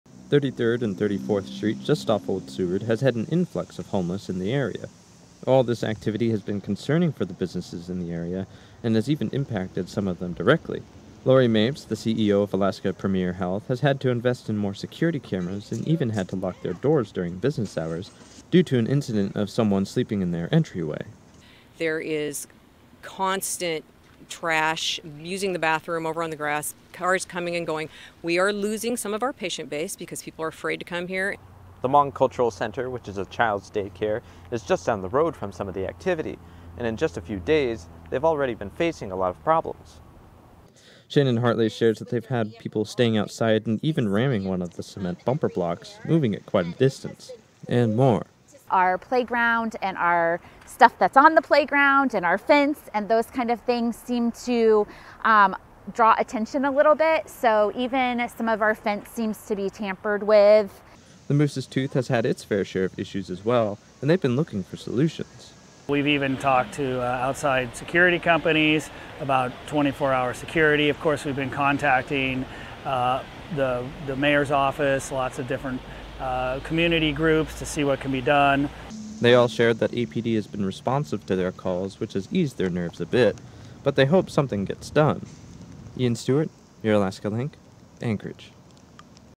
Homeless in Anchorage Settle Near Midtown Businesses Drawing Concern
Following the recent clearance of the Fairbanks Street homeless camp in Anchorage, displaced individuals have moved to the 33rd and 34th Street area near Old Seward, causing concerns for local businesses. The increase in homelessness has led to problems such as vandalism and security issues, prompting businesses like Alaska Premier Health and the Hmong Cultural Center to invest in additional security measures and seek assistance from the mayor’s office and community groups.